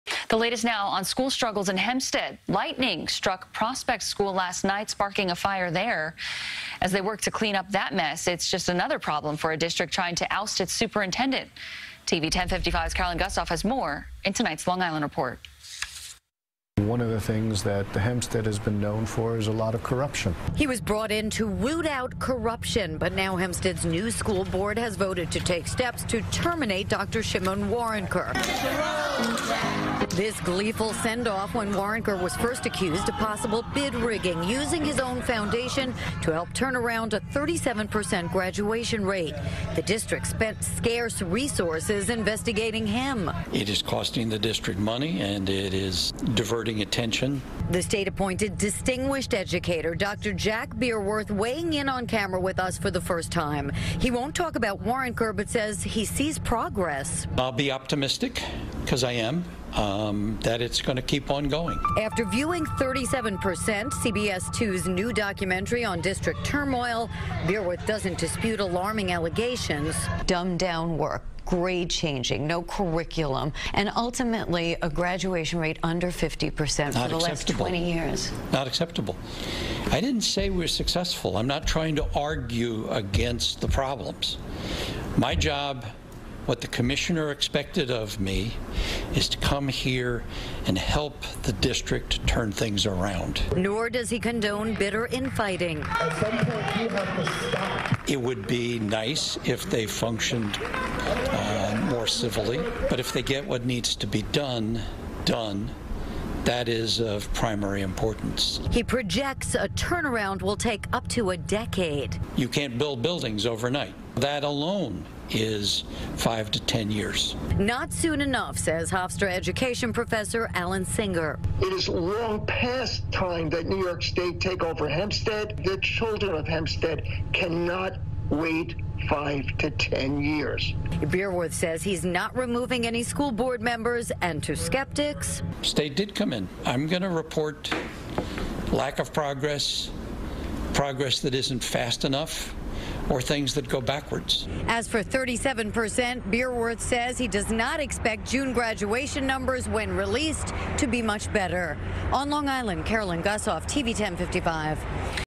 State-Appointed Monitor Talks About Plan For Struggling Hempstead School District
A Long Island school district is moving ahead with steps to dismiss its superintendent.